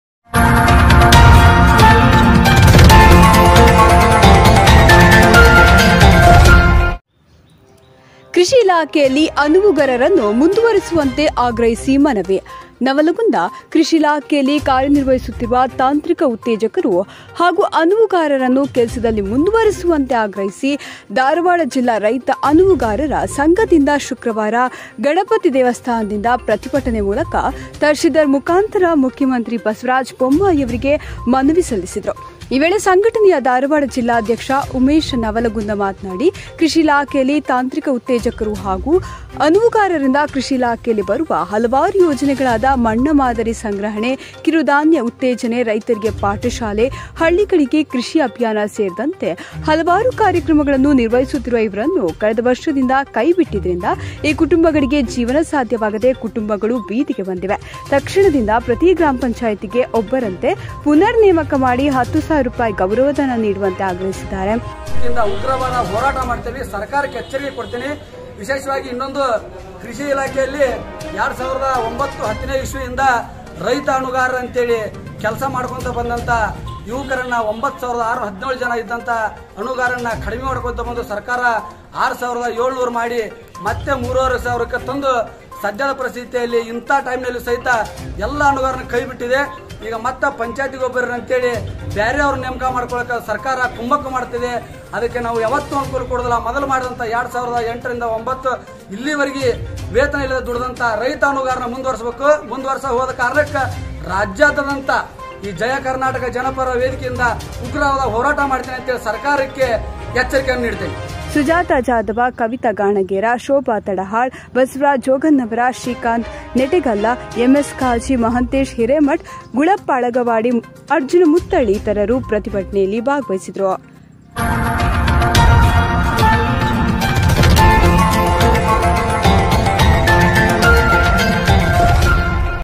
ಕೃಷಿ ಇಲಾಖೆಯಲ್ಲಿ ಅನುವುಗರರನ್ನು ಮುಂದುವರೆಸುವಂತೆ ಅಗ್ರಹಿಸಿ ಮನವಿ
ನವಲಗುಂದ : ಕೃಷಿ ಇಲಾಖೆಯಲ್ಲಿ ಕಾರ್ಯನಿರ್ವಹಿಸುತ್ತಿರುವ ತಾಂತ್ರಿಕ ಉತ್ತೇಜಕರು ಹಾಗೂ ಅನುವುಗಾರರನ್ನು ಕೆಲಸದಲ್ಲಿ ಮುಂದುವರೆಸುವಂತೆ ಆಗ್ರಹಿಸಿ ಧಾರವಾಡ ಜಿಲ್ಲಾ ರೈತ ಅನುವುಗಾರರ ಸಂಘದಿಂದ ಶುಕ್ರವಾರ ಗಣಪತಿ ದೇವಸ್ಥಾನದಿಂದ ಪ್ರತಿಭಟನೆ ಮೂಲಕ ತಹಶೀಲ್ದಾರ್ ಮುಕಾಂತರ ಮುಖ್ಯಮಂತ್ರಿ ಬಸವರಾಜ್ ಬೊಮ್ಮಾಯಿ ಅವರಿಗೆ ಮನವಿ ಸಲ್ಲಿಸಿದರು.

ಈ ವೇಳೆ ಸಂಘಟನೆಯ ಧಾರವಾಡ ಜಿಲ್ಲಾಧ್ಯಕ್ಷ ಉಮೇಶ  ನವಲಗುಂದ ಮಾತನಾಡಿ ಕೃಷಿ ಇಲಾಖೆಯಲ್ಲಿ ತಾಂತ್ರಿಕ ಉತ್ತೇಜಕರು ಹಾಗೂ ಅನುವುಗಾರರಿಂದ ಕೃಷಿ ಇಲಾಖೆಯಲ್ಲಿ ಬರುವ ಹಲವಾರು ಯೋಜನೆಗಳಾದ  ಮಣ್ಣ ಮಾದರಿ ಸಂಗ್ರಹಣೆ ಕಿರುದಾನ್ಯ ಉತ್ತೇಜನೆ ರೈತರಿಗೆ ಪಾಠಶಾಲೆ, ಹಳ್ಳಿಗಳಿಗೆ ಕೃಷಿ ಅಭಿಯಾನ ಸೇರಿದಂತೆ ಹಲವಾರು ಕಾರ್ಯಕ್ರಮಗಳನ್ನು ನಿರ್ವಹಿಸಿತ್ತಿರುವ ಇವರನ್ನು ಕಳೆದ ವರ್ಷದಿಂದ ಕೈ ಬಿಟ್ಟಿದ್ದರಿಂದ ಈ ಕುಟುಂಬಗಳಿಗೆ ಜೀವನ ಸಾಧ್ಯವಾಗದೆ ಕುಟುಂಬಗಳು ಬೀದಿಗೆ ಬಂದಿವೆ.

ತಕ್ಷಣದಿಂದ ಪ್ರತಿ ಗ್ರಾಮ ಪಂಚಾಯತಿಗೆ ಒಬ್ಬರಂತೆ ಪುನರ್ ನೇಮಕ ಮಾಡಿ ಹತ್ತುಸಾವಿರ ರೂ ಗೌರವಧನ ನೀಡುವಂತೆ ಅಗ್ರಹಿಸಿದ್ದಾರೆ.

ಸುಜಾತಾ ಜಾಧವ, ಕವಿತಾ ಗಾಣಗೇರ, ಶೋಭಾ ತಡಹಾಳ್, ಬಸವರಾಜ್ ಜೋಗನ್ನವರ, ಶ್ರೀಕಾಂತ್ ನೆಟೆಗಲ್ಲ, ಎಂ ಎಸ್ ಖಾಜಿ, ಮಹಾಂತೇಶ್ ಹಿರೇಮಠ್, ಗುಳಪ್ಪ ಅಳಗವಾಡಿ, ಅರ್ಜುನ ಮುತ್ತಳ್ಳಿ ಇತರರು ಪ್ರತಿಭಟನೆಯಲ್ಲಿ ಭಾಗವಸಿದ್ದರು.